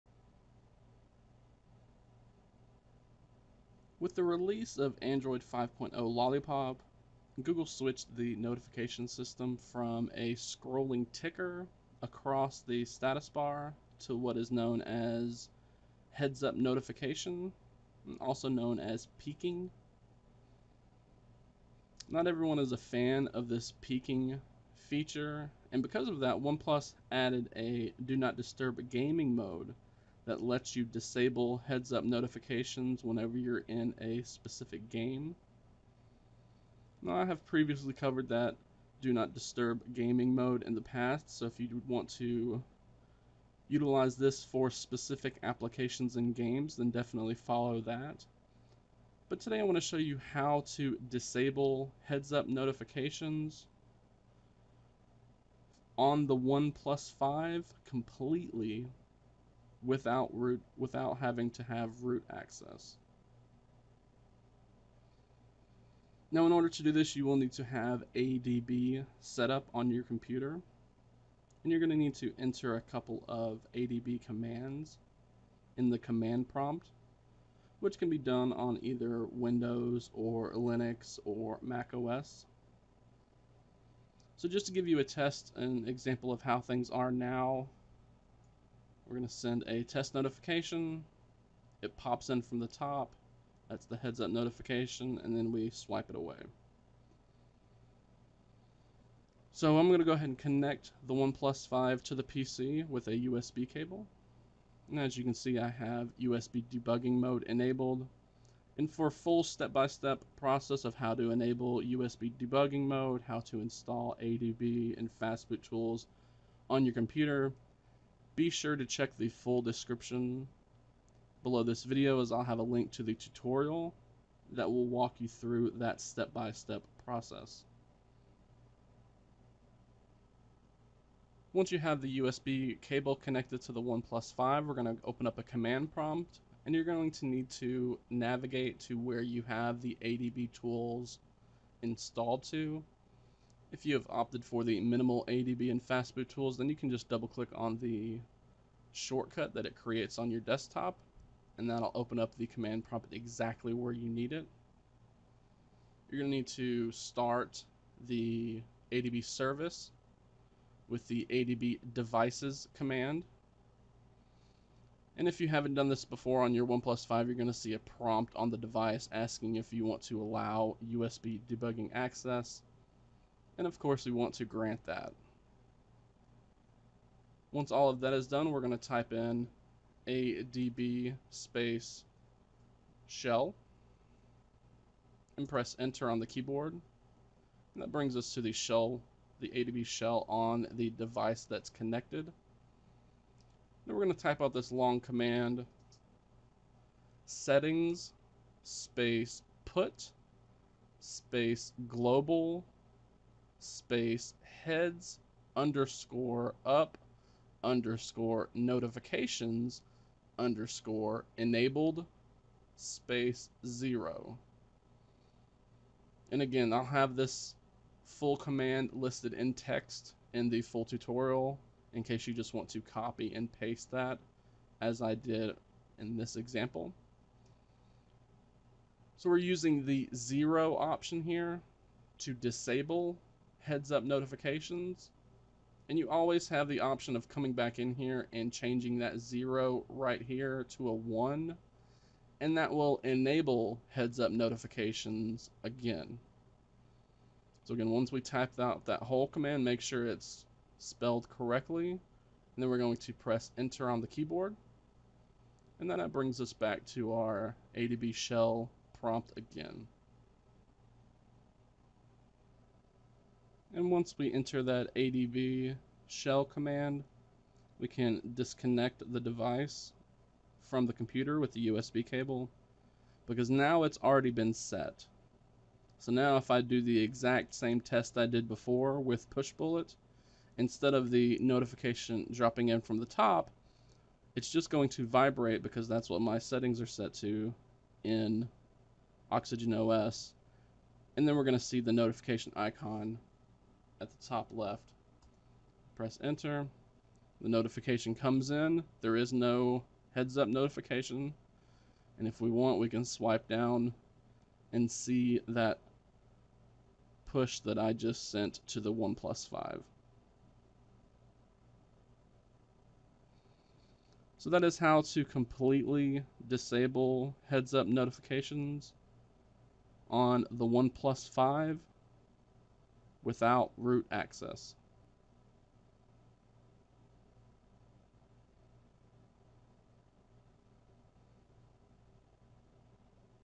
OnePlus 5 | Disable Heads Up Notifications
OnePlus 5 owners can follow this simple step by step guide to complete disable heads-up notifications (which are also known as peeking notifications).